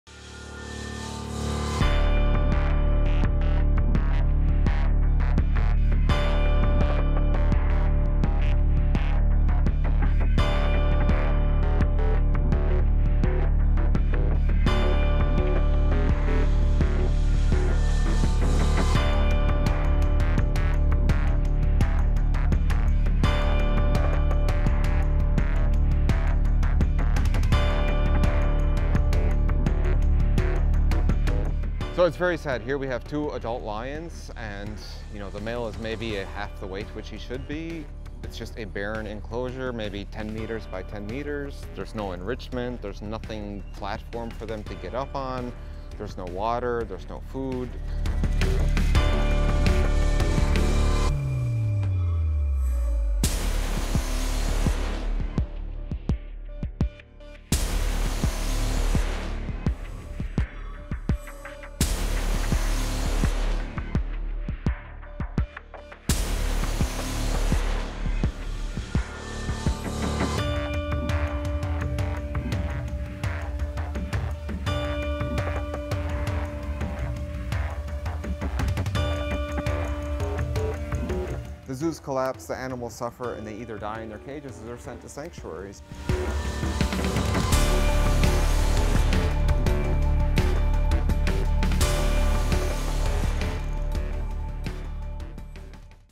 الجوع يطال الحيوانات في لبنان
بسبب الأزمة الاقتصادية الخانقة التي تضرب لبنان.. الجوع يطال الحيوانات